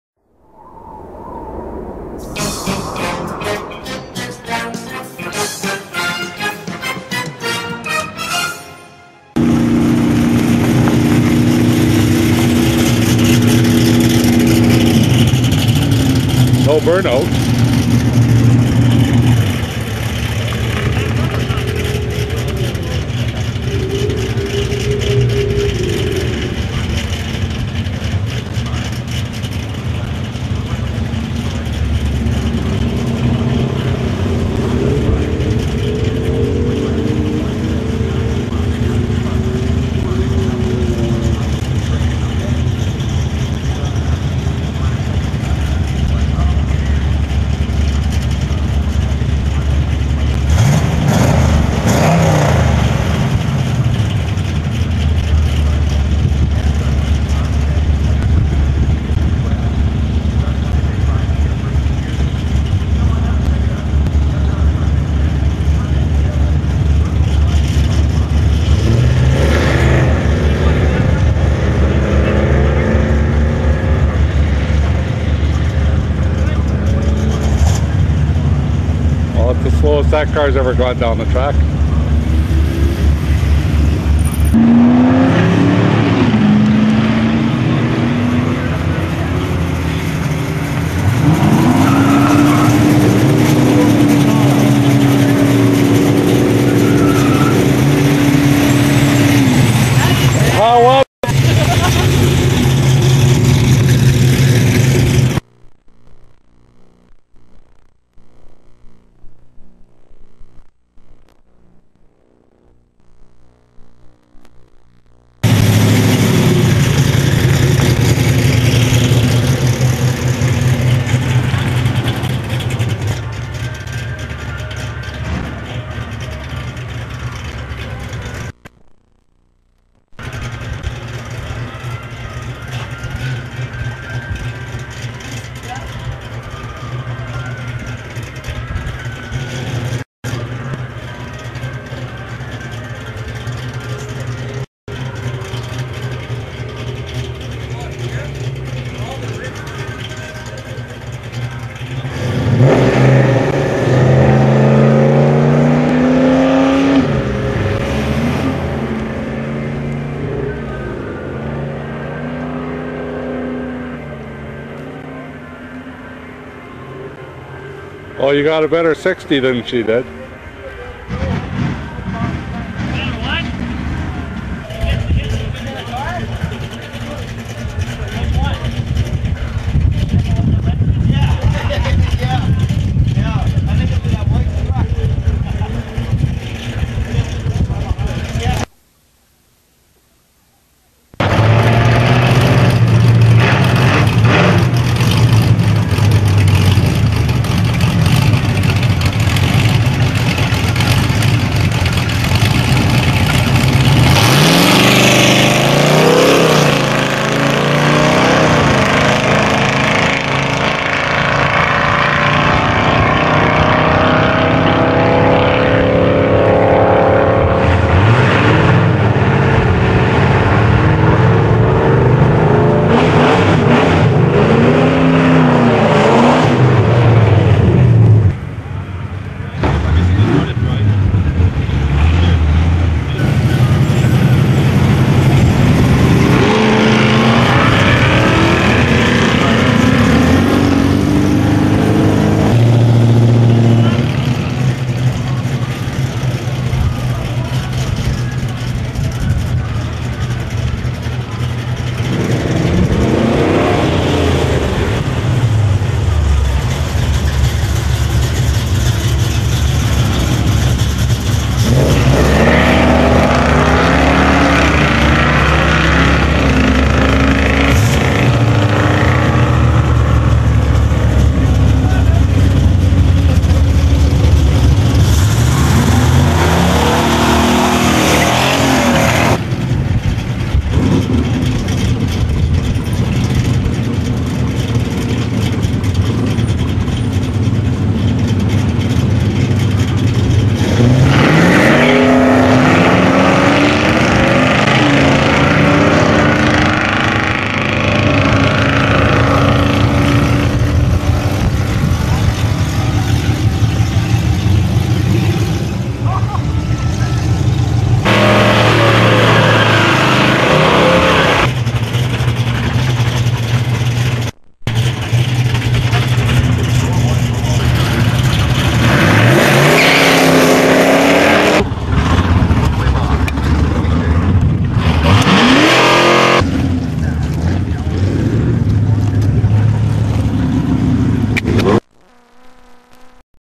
Janina & Colleen Racing
My wife's first drag race. She races Colleen who is also having her first drag race. Colleen is running Daryls  (Downtown Brown)  Monte. This was in 2001 prior to the Monte joining weight watchers and lost a lot of metal.Janina is in our race track before all he frame and body mods. The engine was a 383 stroker 400 hp 400 ft/lbs of torque. on her first run she did 97mph